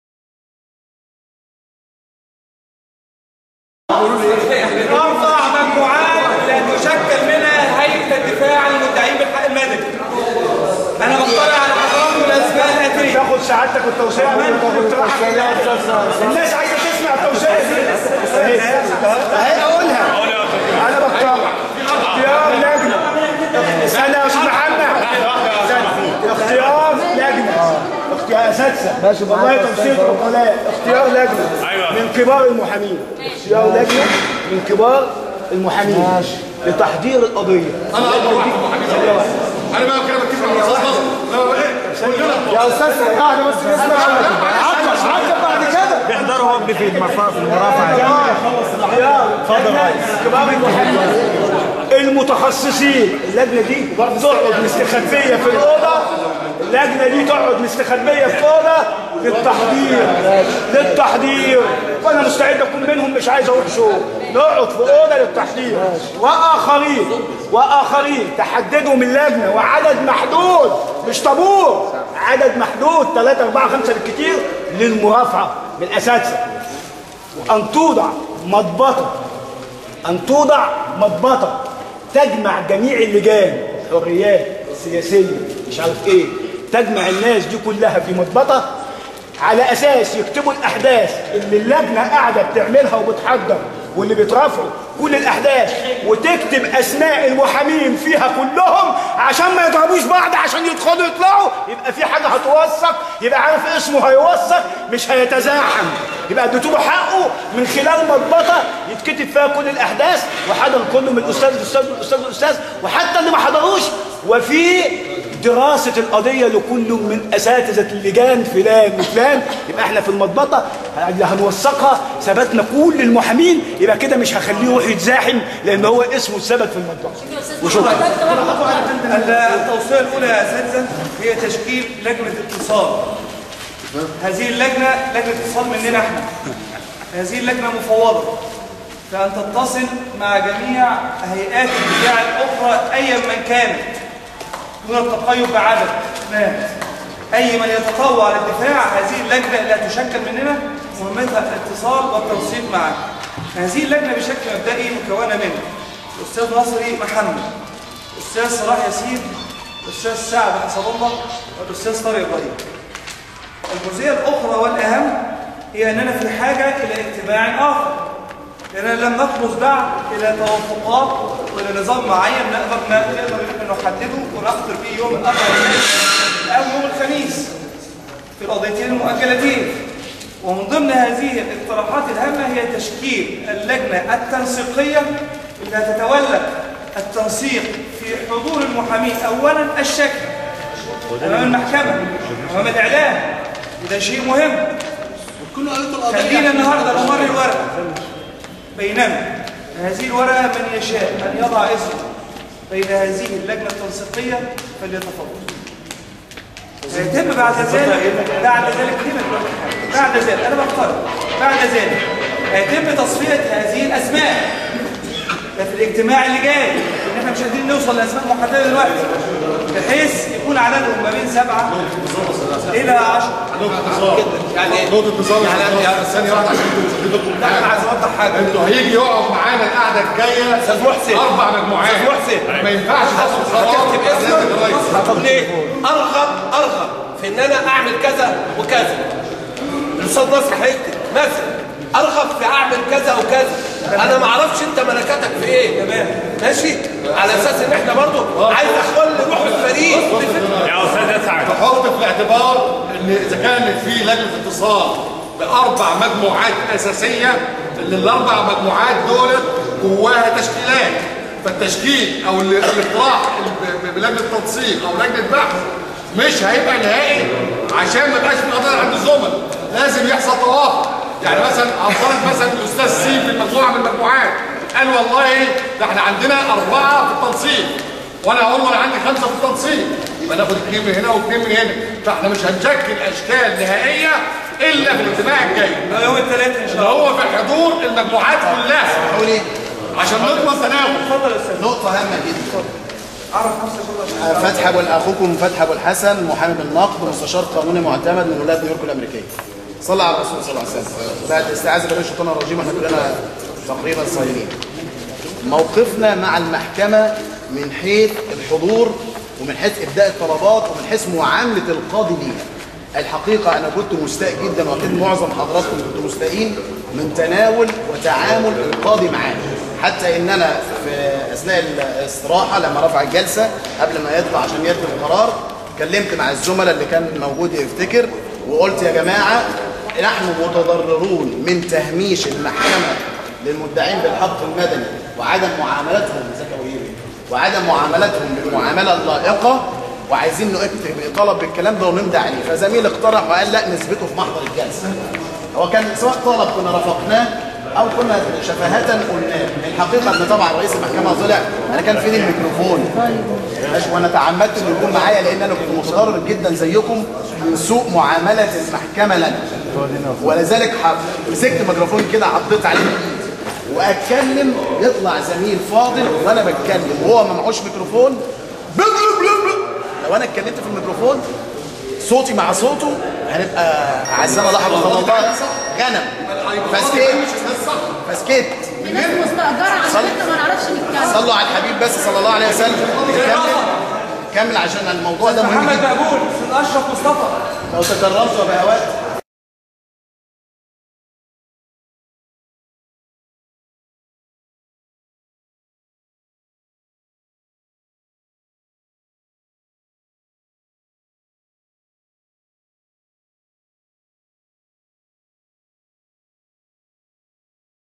أراء المحاميين و إستعدادتهم لمحاكمة مبارك
فى إجتماع لعدد من المحامين بلجنة الشئون السياسية بنقابة المحاميين إستعداداً لمحاكمة مبارك أراء و مناقشات بينهم